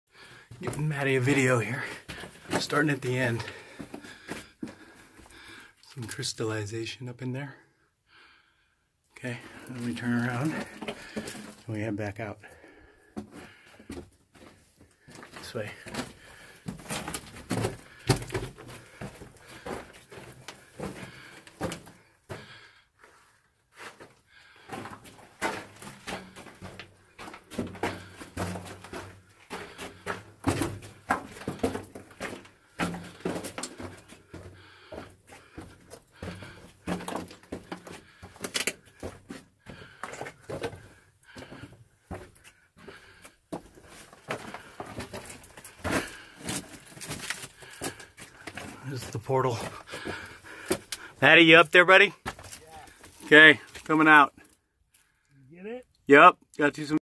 #3 Blue Jay Jack Rabbit mine inside tunnel 2nd entry
This is my 2nd entry into our Blue Jay Jack Rabbit mine tunnel that I had just discovered hours earlier. I re-entered the tunnel, just after exiting for the first time, with my brother Matt's phone to capture some video for him. The audio quality is much better than my initial GoPro video.